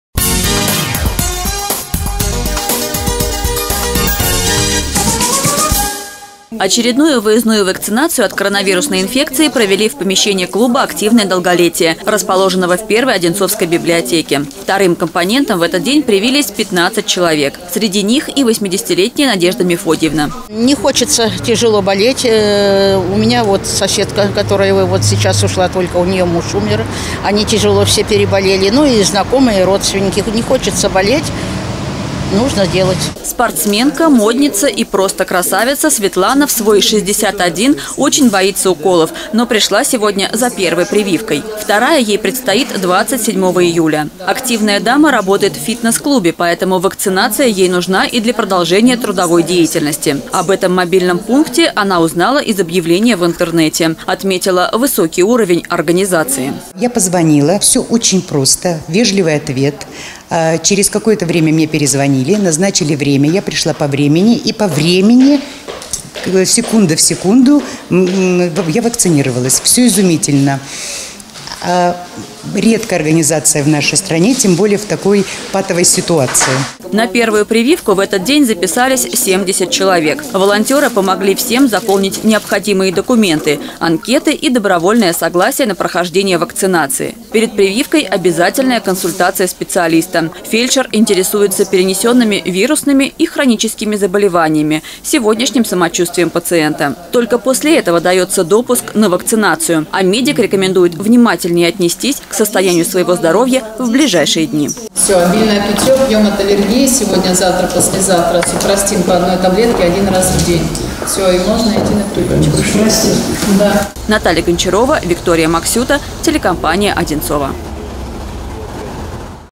Выездная вакцинация в Библиотеке №1
В одинцовском клубе «Активное долголетие» провели очередную выездную вакцинацию от коронавирусной инфекции. Первый компонент вакцины смогли получить 70 представителей старшего поколения, второй – 15 человек.